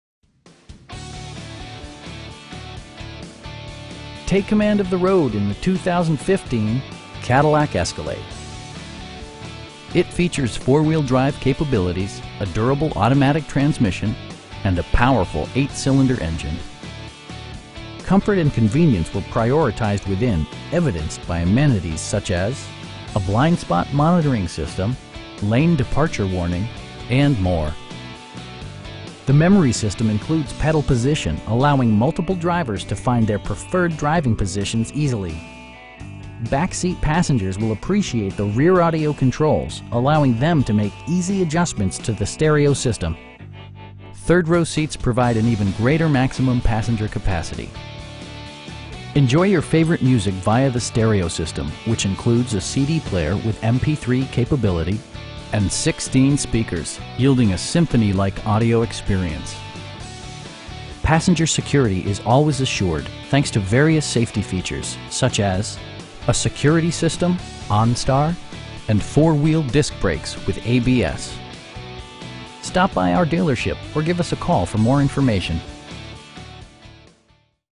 2015 CADILLAC ESCALADE Luxury in Bentonville, AR 72712
CADILLAC OF BENTONVILLE
2300 SE Moberly Lane

Take command of the road in the 2015 CADILLAC ESCALADE. It features four-wheel drive capabilities, a durable automatic transmission, and a powerful 8 cylinder engine. Comfort and convenience were prioritized within, evidenced by amenities such as: leather upholstery, an automatic dimming rear-view mirror, a blind spot monitoring system, front and rear air conditioning, power moon roof, lane departure warning, and more. The memory system includes pedal position, allowing multiple drivers to find their preferred driving positions easily. Back seat passengers will appreciate the rear audio controls, allowing them to make easy adjustments to the stereo system. Rear passengers enjoy the seat heating functionality, keeping them warm during the winter months. Third row seats provide an even greater maximum passenger capacity. Rear LCD monitors provide entertainment that your passengers will appreciate no matter how far the drive. Passenger security is always assured thanks to various safety features, such as: dual front impact airbags with occupant sensing airbag, front SIDE impact airbags, traction control, brake assist, a security system, onStar, and 4 wheel disc brakes with ABS. You'll never lose visibility with rain sensing wipers, which activate automatically when the drops start to fall. Our aim is to provide our customers with the best prices and service at all times. Stop by our dealership or give us a call for more information.